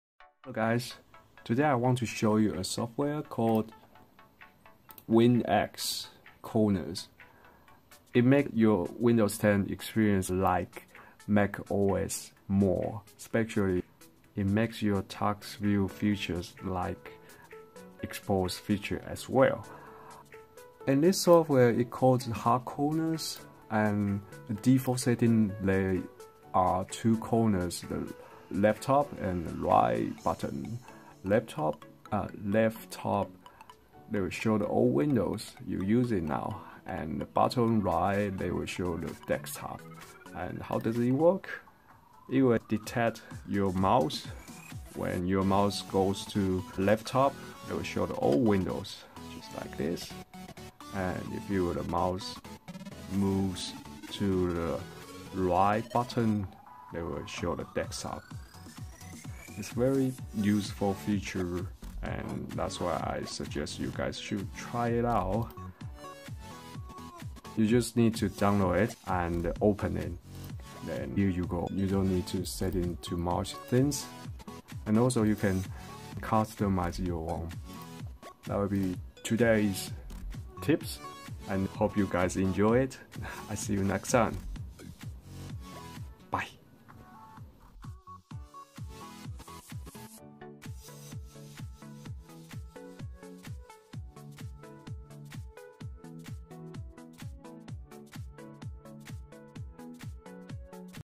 WinXCorner - Best Win 10 TaskView add-on Software
demoing WinXcorners, makes your windows 10 more productive